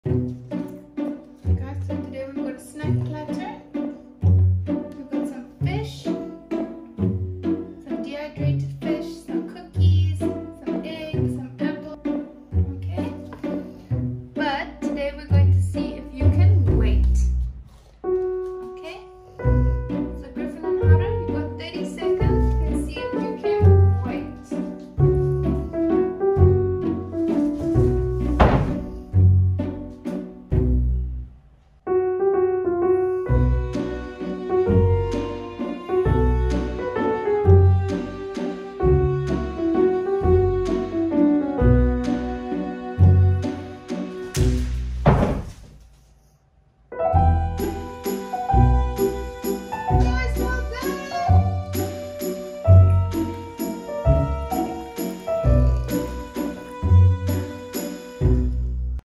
Hungry Dogs TRY To Resist Snack Platter
French Bulldogs try the leave it challenge or good boy challenge. Leaving my French Bulldogs alone with their favorite snacks! Can Griffin and Haru resist? See if they do or don't in today's temptation test.